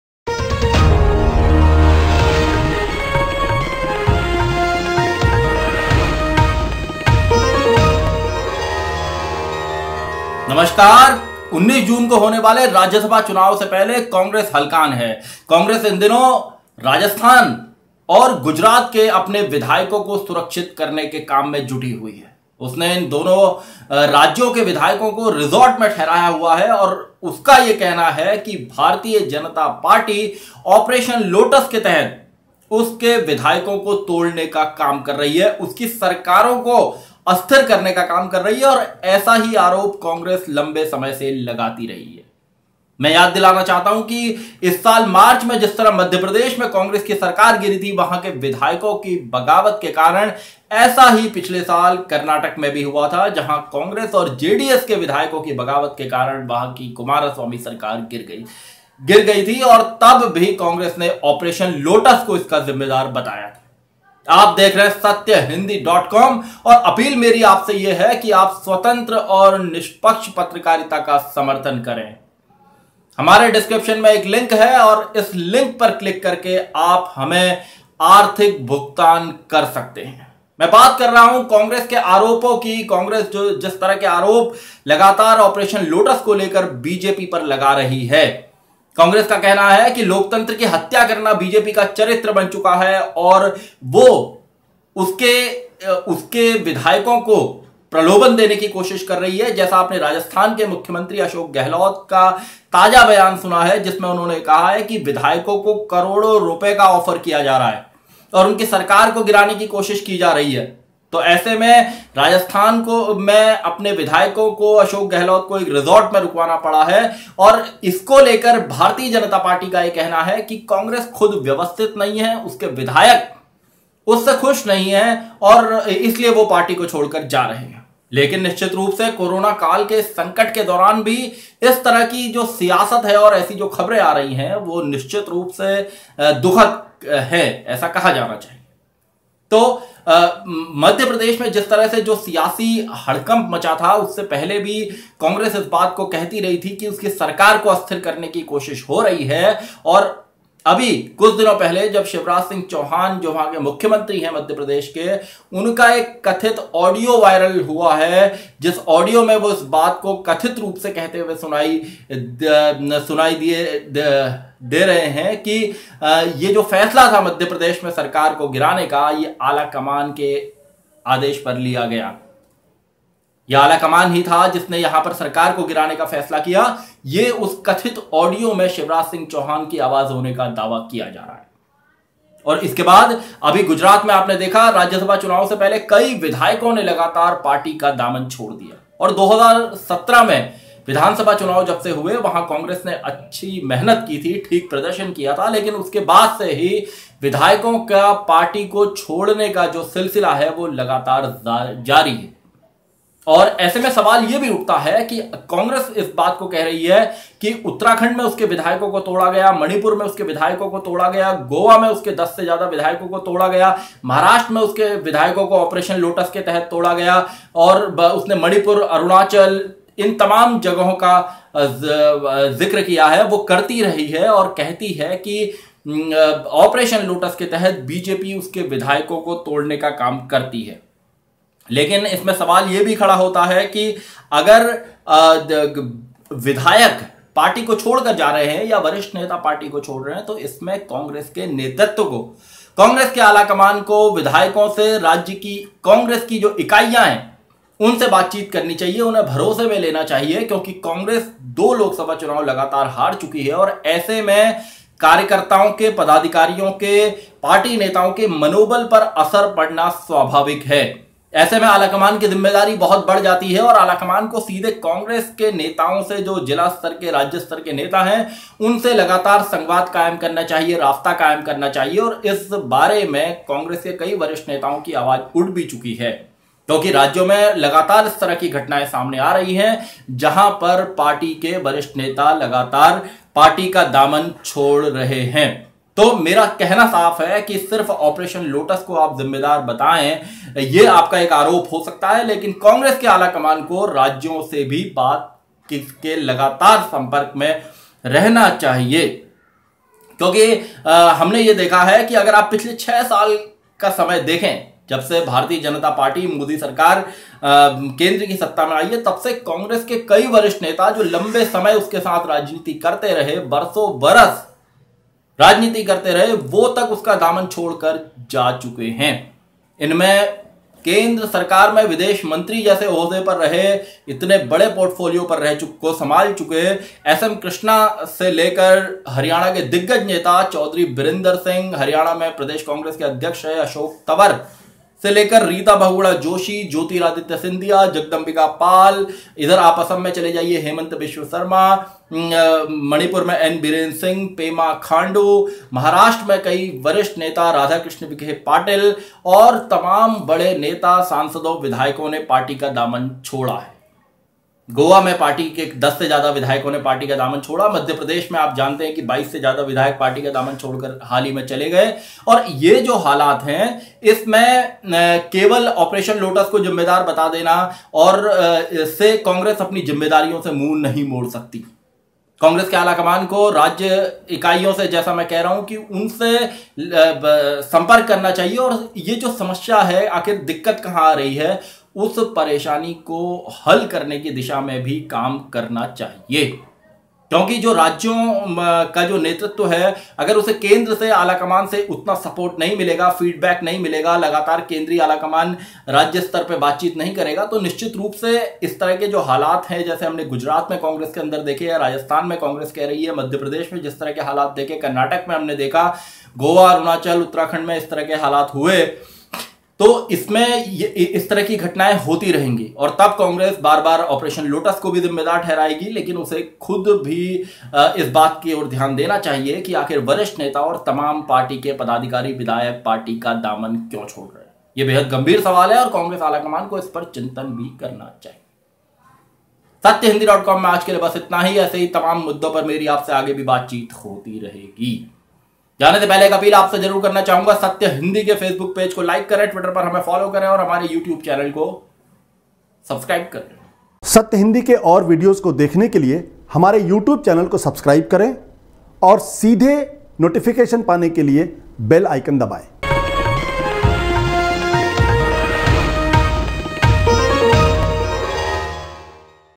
हमारी सरकारों को गिराने की कोशिश: कांग्रेस
19 जून को होने वाले राज्यसभा चुनाव से पहले कांग्रेस हलकान है। वह गुजरात, राजस्थान के अपने विधायकों को सुरक्षित करने के लिए पसीना बहा रही है। कांग्रेस का कहना है कि उसके विधायकों को तोड़ने और उसकी सरकारों को गिराने के पीछे कारण बीजेपी का ‘ऑपरेशन लोटस’ है।